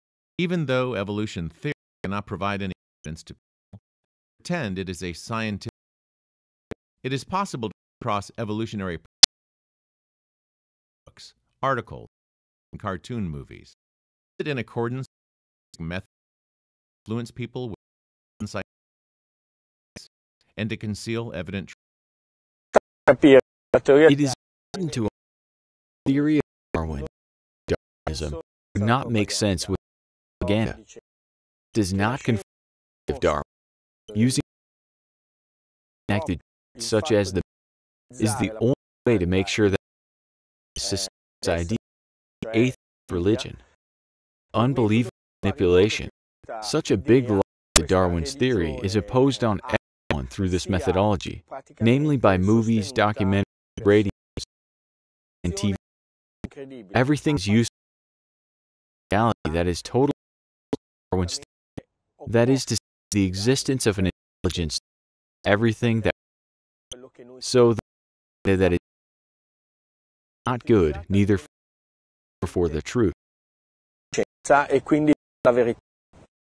Dr. Fabrizio Fratus: Is it in accordance with scientific methods to...
Even though evolution theory cannot provide any evidence to people, they pretend it is a scientific fact. It is possible to come across evolutionary propaganda in movies, text books, articles and even cartoon movies. Is it in accordance with scientific methods to try to influence people with various unscientific propaganda methods and to conceal evident truths?

Fabrizio: It is important to understand that the theory of Darwin and neo-Darwinism would not make sense without the propaganda. Science does not confirm the theory of Darwin. Using propaganda and the means connected to it such as the media is the only way to make sure that people in practice sustain this idea, this atheist religion. This is an unbelievable manipulation. Such a big lie, the Darwin's theory, is imposed on everyone through this methodology, namely by movies, documentaries, radios, interviews and TV channels. Everything is useful to falsify the reality that is totally opposed to Darwin's theory, that is to say the existence of an intelligence that produced everything that we are. So, the propaganda that is used is absolutely not good neither for science, nor for the truth.